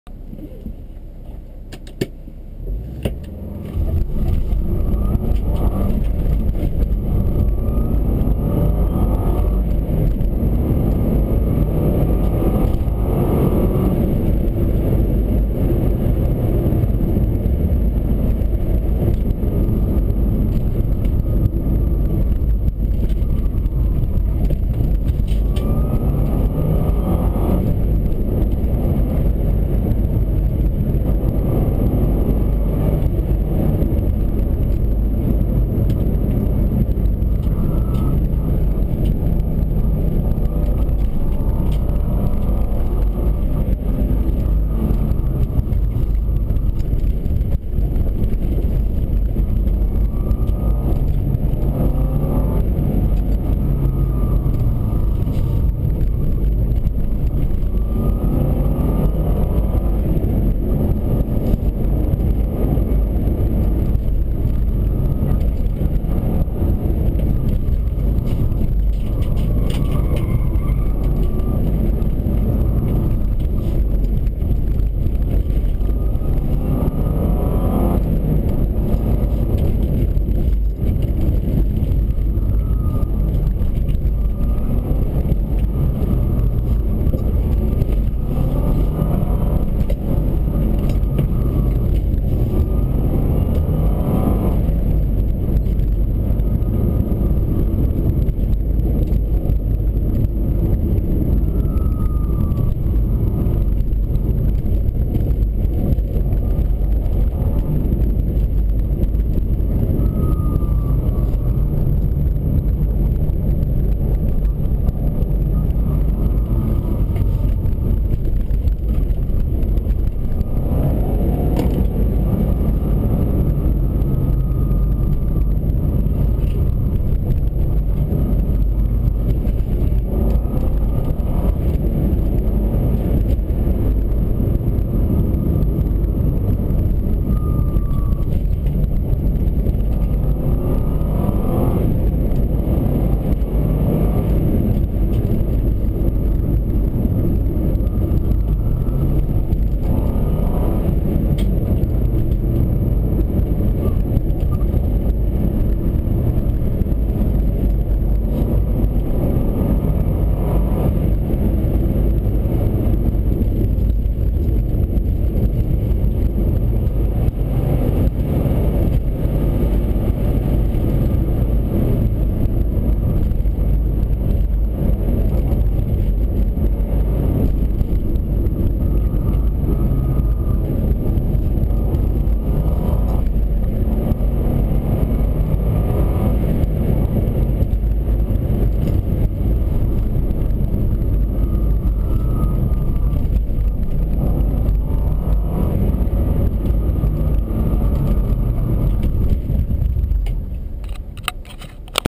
Forester Country 2
A little cruise in my 03 Forester, lots of fun!